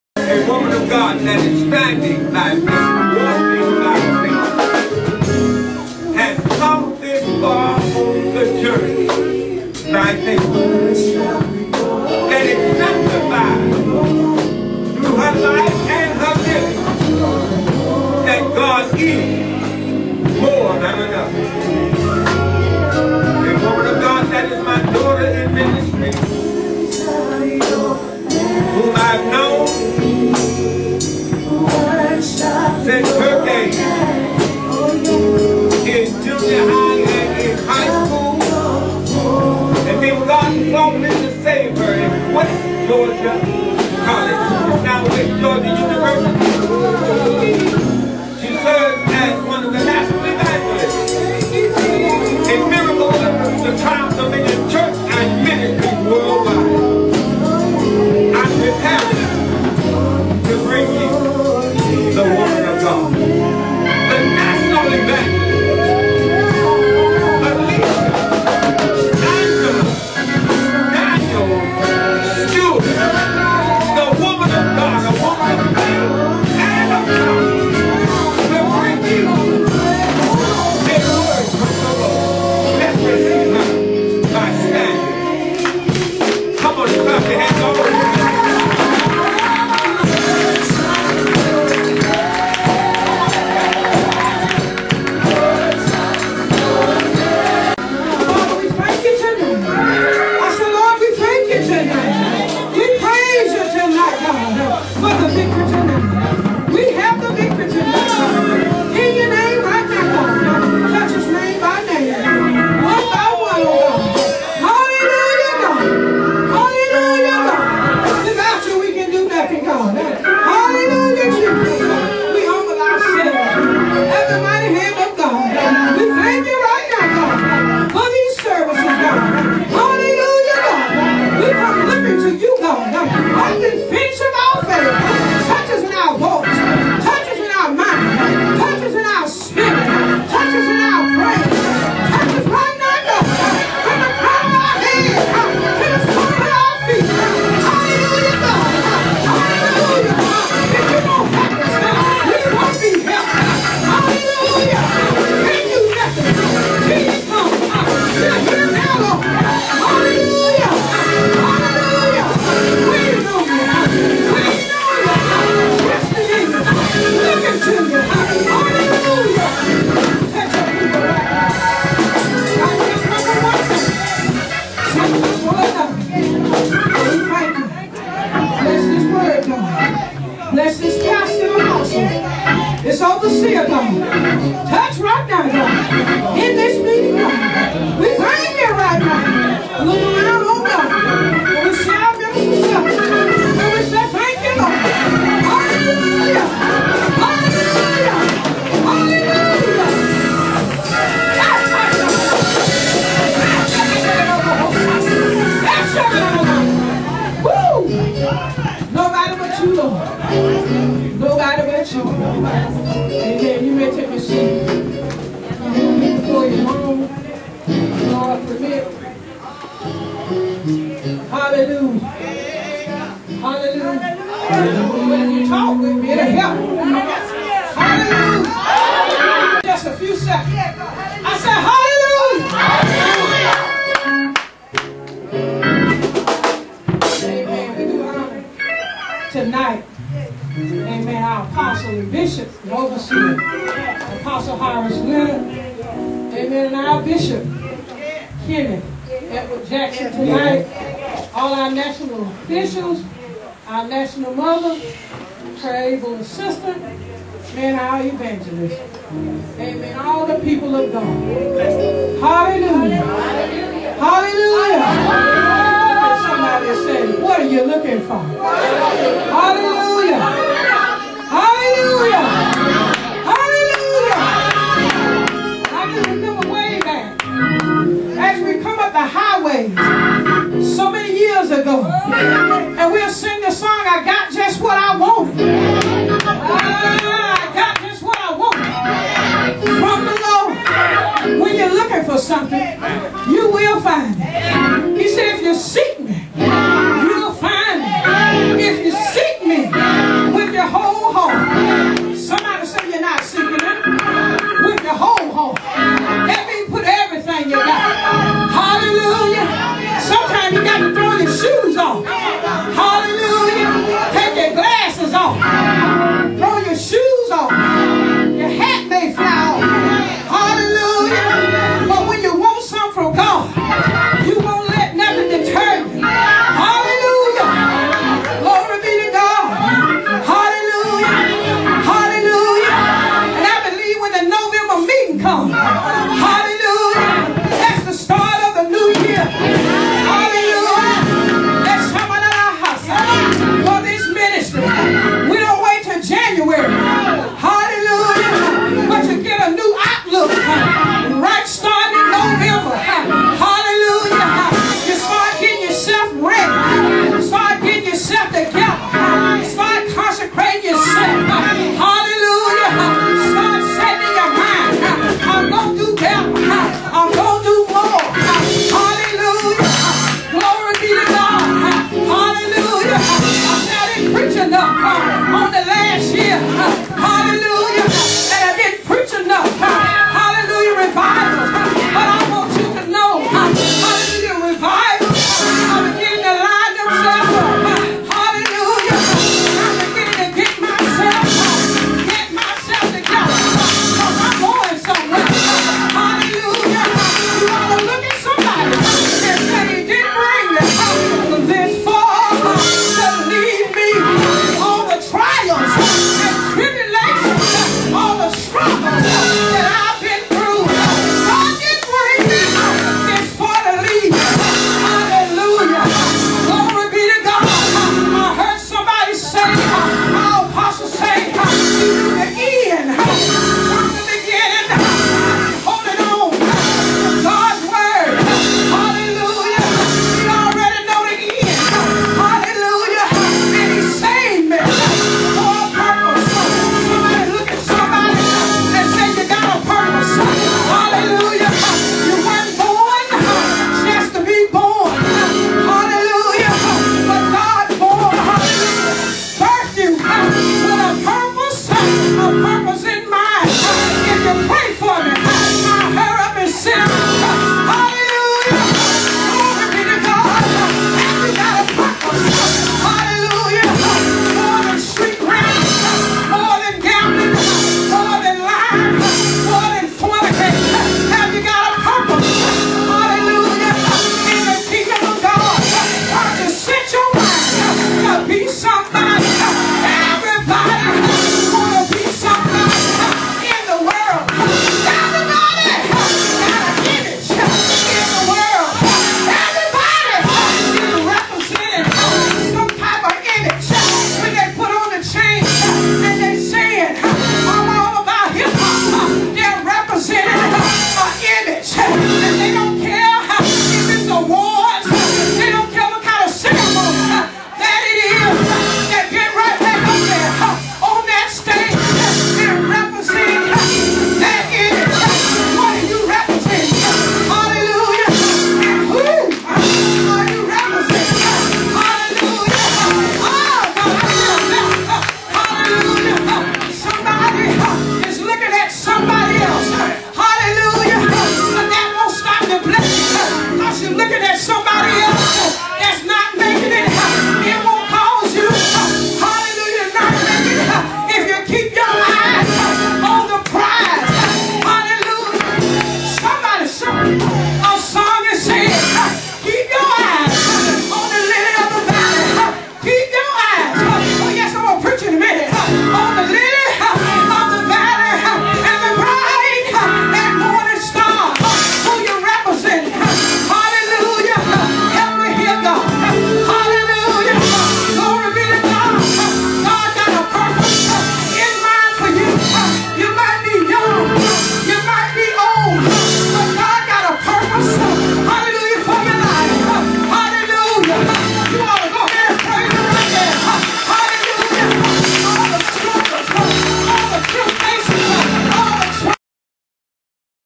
MDTDCMW-Evang. Stewart (Surely)
Evang. Stewart preaching from the topic, "Surely".
Scripture Text: Isaiah 53:4-5